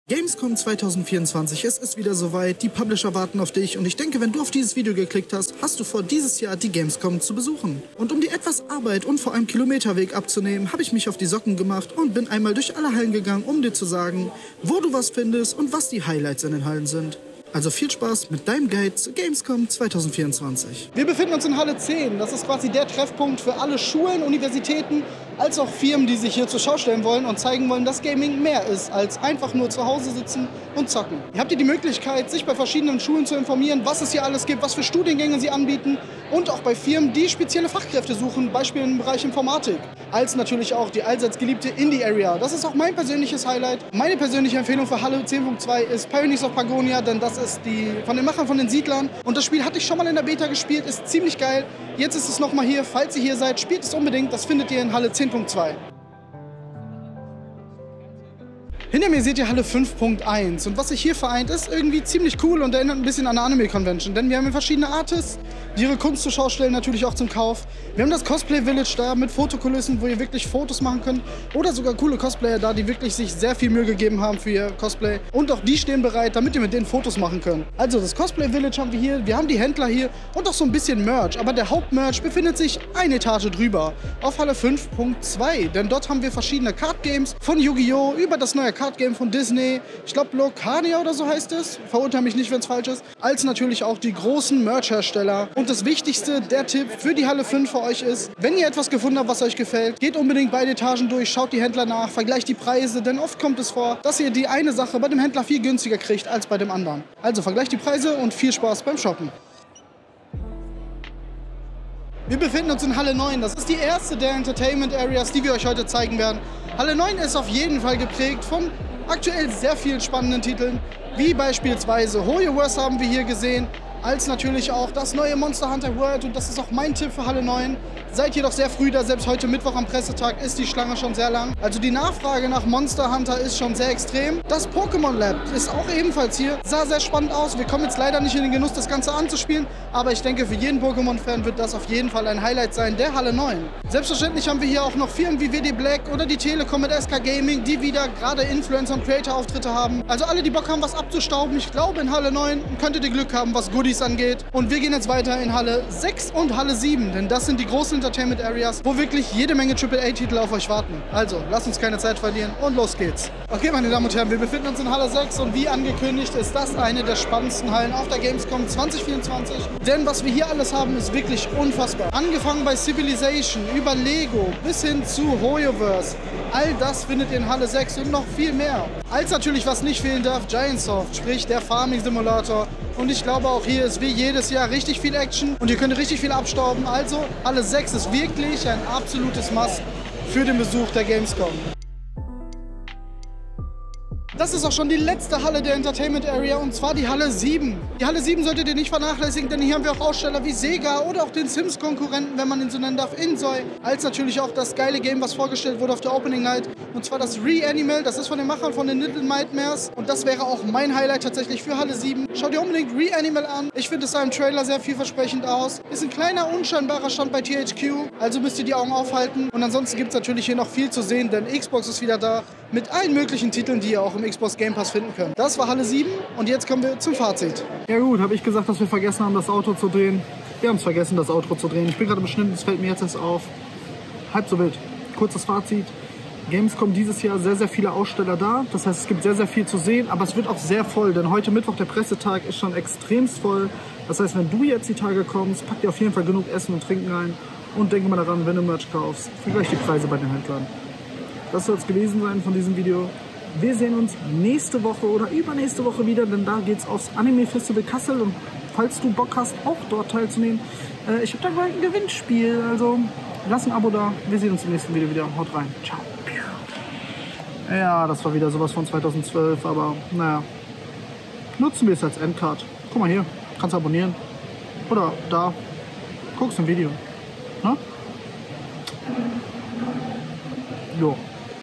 Gamescom 2024 - Das sind die Highlights!
Wenn du dieses Jahr die Gamescom besuchst, dann solltest du dieses Video vorher ansehen um keins der Highlights der Gamescom zu verpassen!
Ansonsten wünsche ich viel spaß mit dem Rundgang der Gamescom 2024.